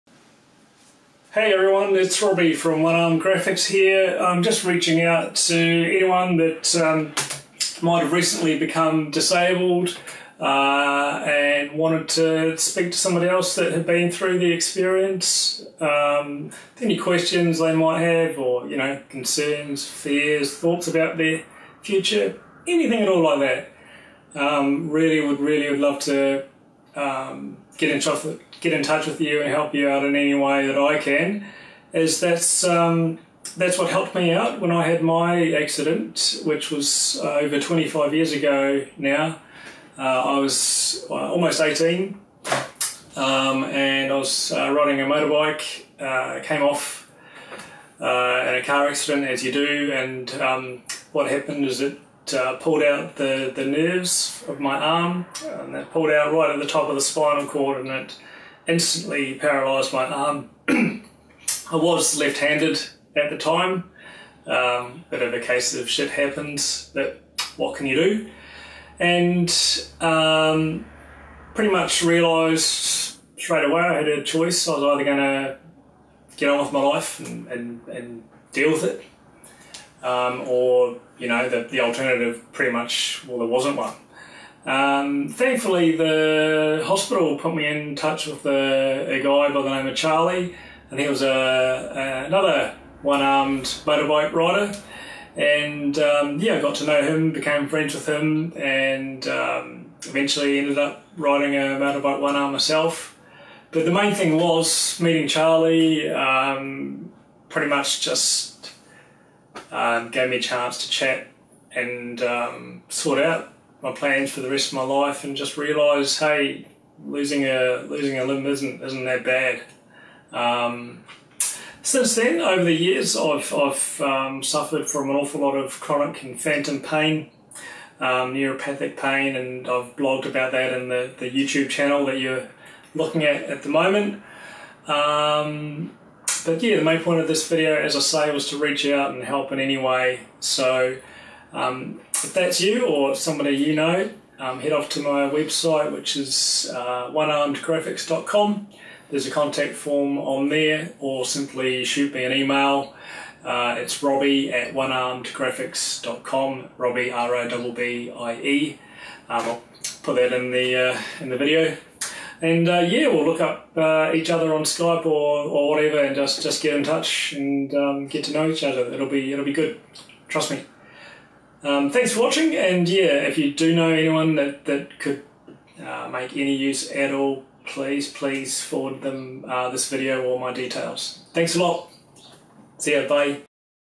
One-armed Mentor
When I had my motorbike accident 25 years ago, that paralysed my dominant arm and gave me a few other bumps and grazes, one of the best things that happened shortly after was the Hospital introducing me to another one-armed Biker by the name of Charlie.

This video is dedicated to Charlie, who took me under his one good wing, and showed me life as a one-armer was great. He still rode Bikes like a one-armed fool and lived a pretty happy life.

If I can help anyone else who has become disabled in any way, by email or Skype I'd love to hear from you. If you know someone that might benefit please send them a link to this page, or the video.

Lets get in touch and answer all those weird questions and put your mind at rest.

I was left-handed and my best mate Blake, casually strolled into the hospital room for the first time and took one look and me and cheerfully exclaimed, right in front of the Nurse, "Well... guess you'll be learning to wank with the right-hand now, eh mate?!"